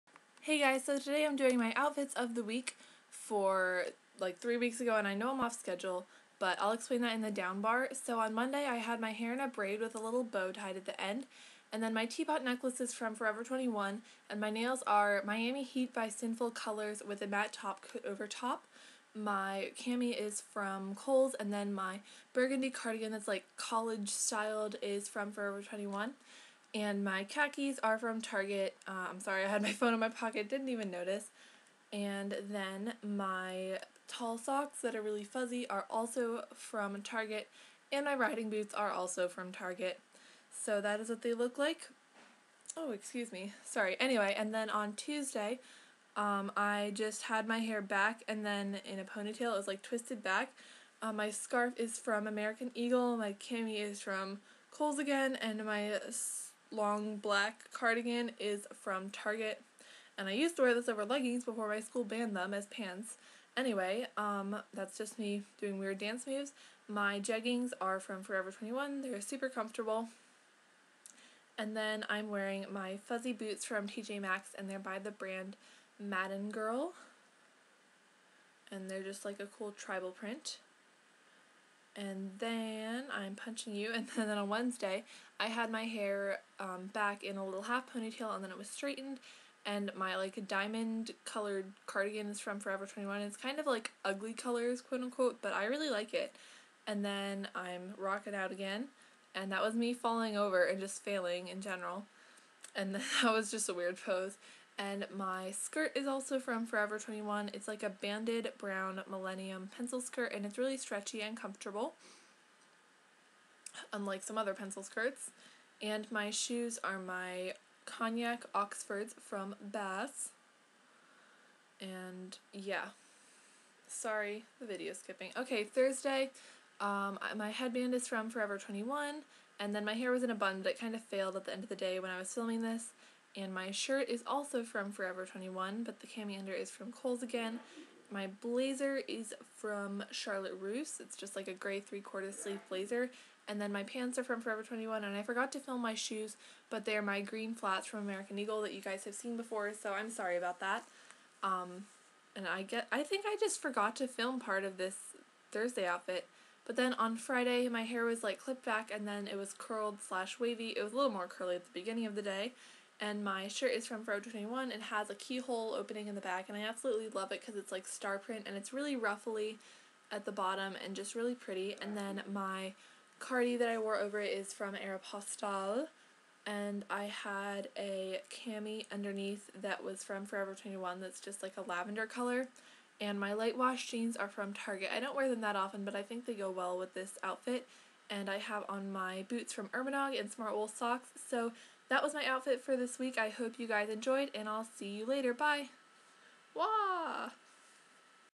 Outfits of the Week: January 7-11
Hey guys, pleases read the downbar; it's important! Thanks :)

Sooo this OOTW is a few weeks late...I know, and I'm sorry. I've been filming my outfits; just haven't had the time to edit and upload them. I will catch up pretty soon, pinky promise!

Outfit details:

Monday
Necklace: Forever21
Cami: Kohl's
Cardi: Forever21
Pants: Target
Socks: Target
Boots: Target
Nails: Sinful Colors "Miami Heat" + Sephora by OPI Matte Topcoat

Tuesday
Scarf: American Eagle
Cami: Kohl's
Cardi: Target
Jeggings: Forever21
Boots: TJMaxx (Brand = Madden Girl)

Wednesday
Sweater: Forever21
Skirt: Forever21
Oxfords: Bass

Thursday
Headband: Forever21
Shirt: Forever21
Cami: Kohl's
Blazer: Charlotte Russe
Pants: Forever21
Flats: Thrift stoer (Brand = American Eagle)

Friday
Shirt: Forever21
Cami: Forever21
Cardi: Aeropostale
Jeans: Target
Socks: Costo
Boots: Urbanog.com